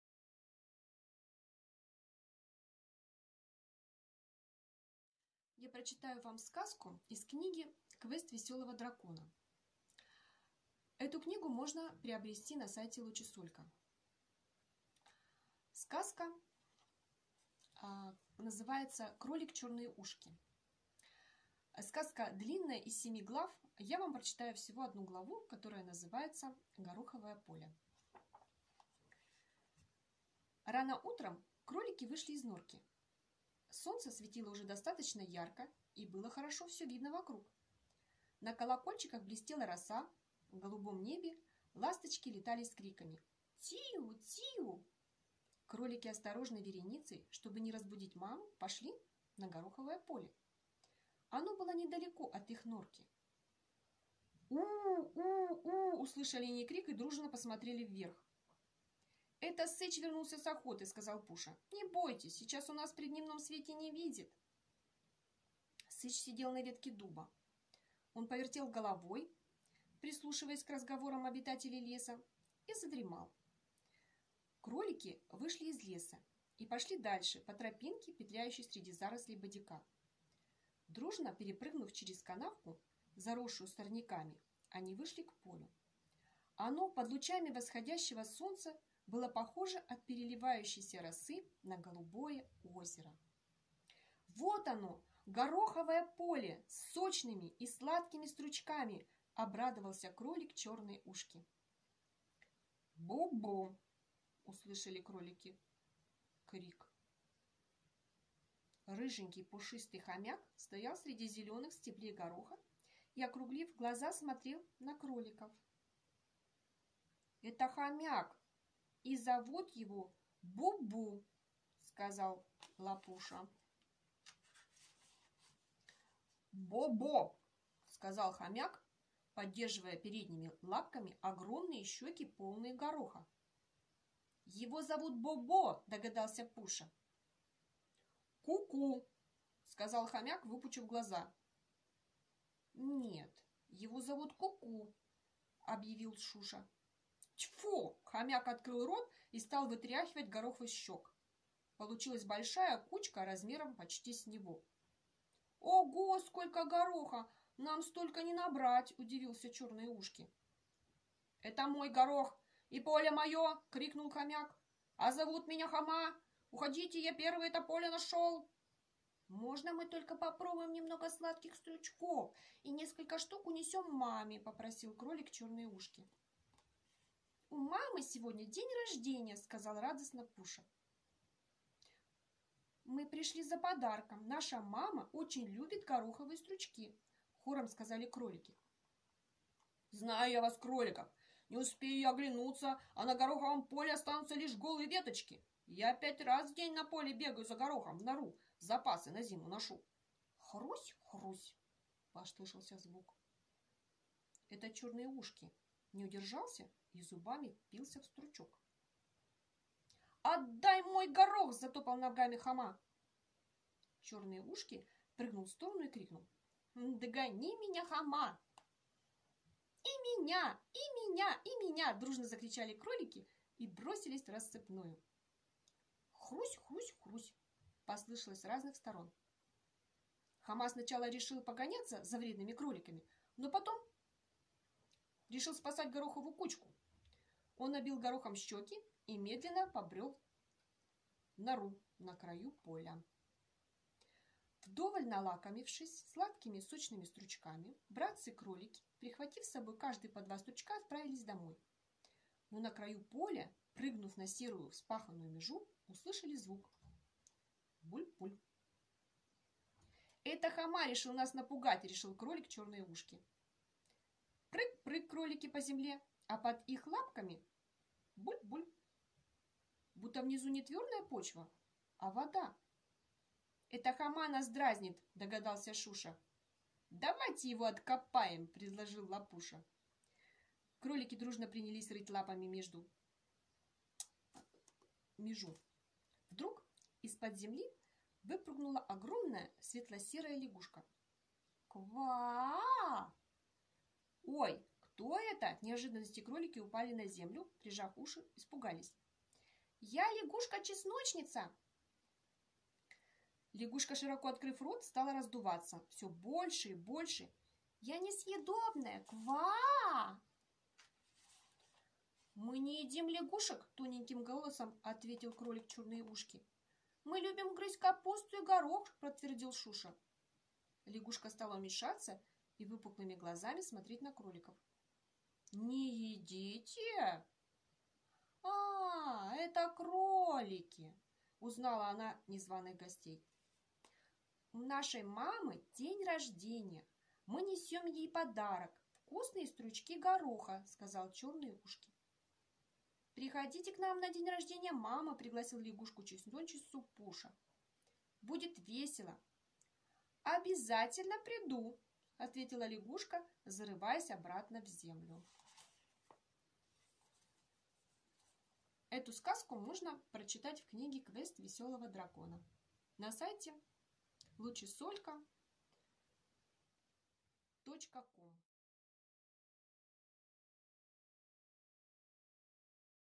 Гороховое поле. Глава из сказки "Кролик Чёрные ушки"
Сказку читает автор. Купите книгу сказок для детей